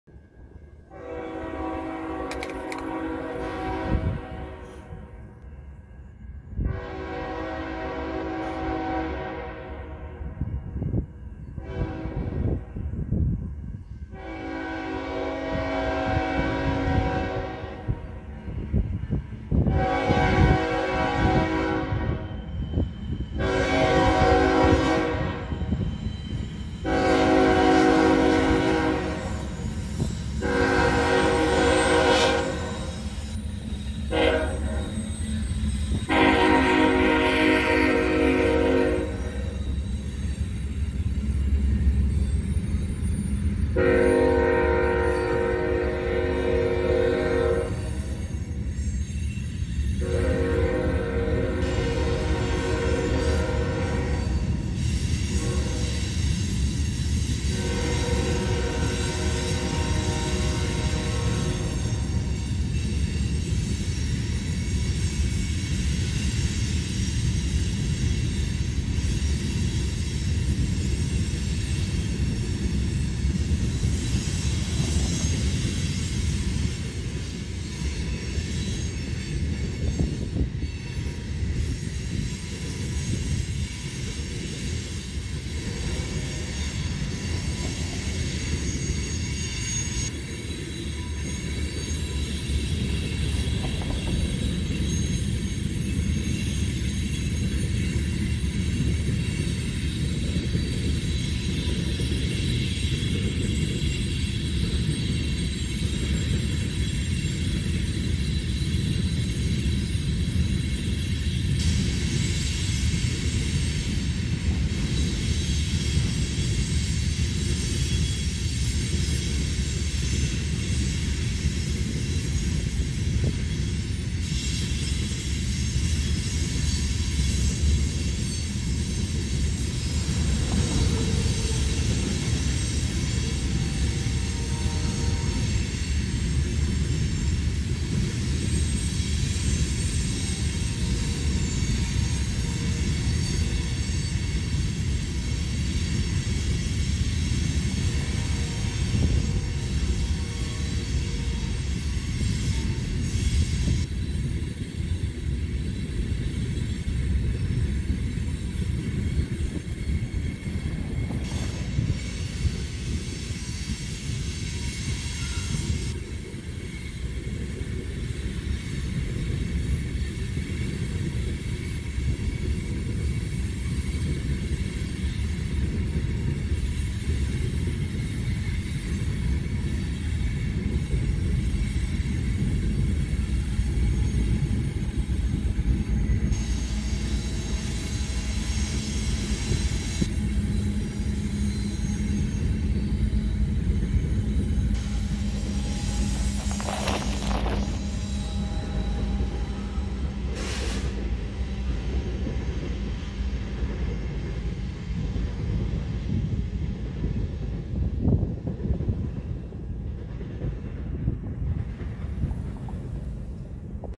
RailFan 017 BNSF train Oh look, more sand...?
June 29, 2021 at Fort Collins

This is a loud one.  More two bay hoppers rolling up to North Yard.  There’s a pink one in there, if you are keeping track.  Those buildings are apartments and the trains run any old time of day.  Try sleeping through that.

I didn’t catch the engine numbers.